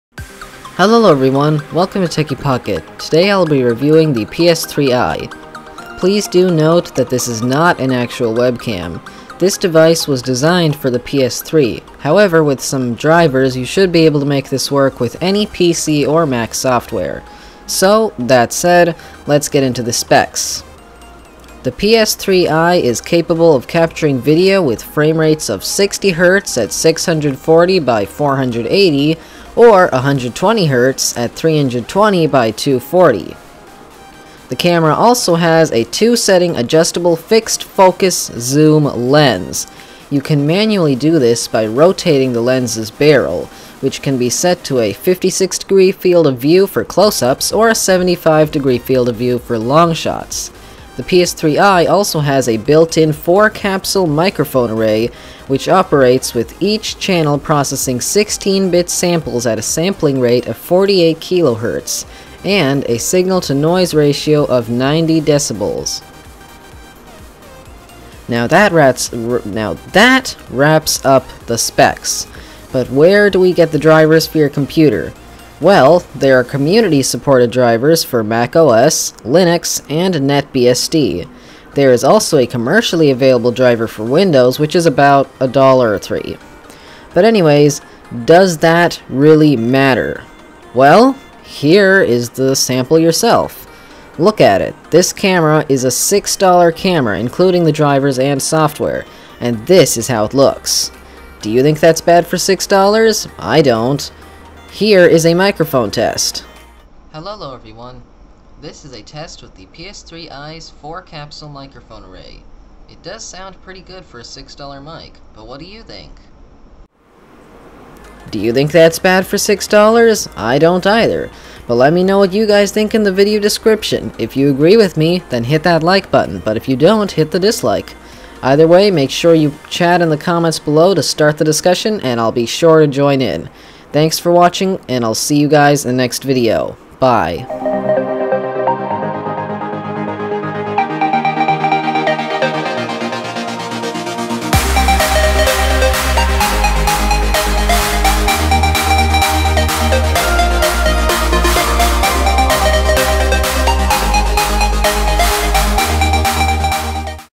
PS3Eye | Value Option or Crappy Edition?
Helolo everyone! Today I will be reviewing the PS3Eye. Even though this is a pretty old camera, its still a very budget option for those looking for a good camera.